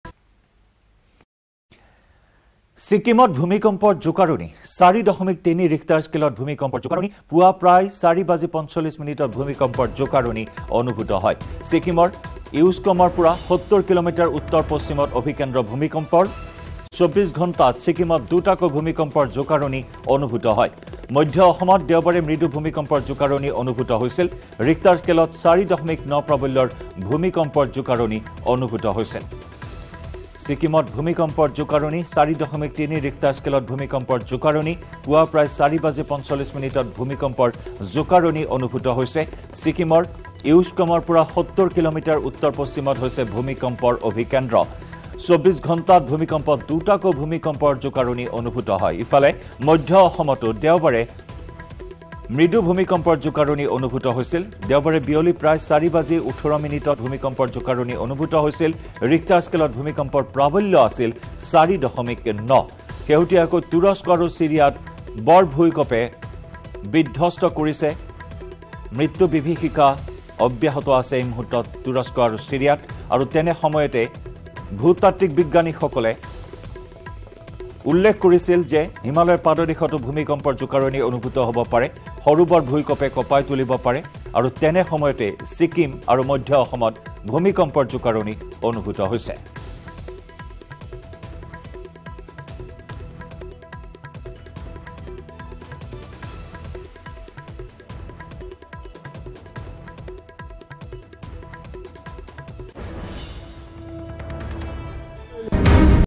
Sikkim Assam Earthquake : ৪.৩ ৰিখটাৰ স্কেলত ভূমিকম্পৰ জোকাৰণি । Earthquake
ছিকিমত ভূমিকম্পৰ জোকাৰণি।
৪.৩ ৰিখটাৰ স্কেলত ভূমিকম্পৰ জোকাৰণি।
পুৱা ৪:৪৫ বজাত ভূমিকম্পৰ জোকাৰণি।
ছিকিমৰ ইউক্সমৰ পৰা ৭০ কিঃমিঃ উত্তৰ-পশ্চিমত অভিকেন্দ্ৰ।
২৪ ঘণ্টাত ছিকিমত দুটাকৈ ভূমিকম্পৰ জোকাৰণি।
মধ্য অসমত দেওবাৰে মৃদু ভূমিকম্পৰ জোকাৰণি।
ৰিখটাৰ স্কেলত ৪.৯ প্ৰাবল্যৰ ভূমিকম্প অনুভূত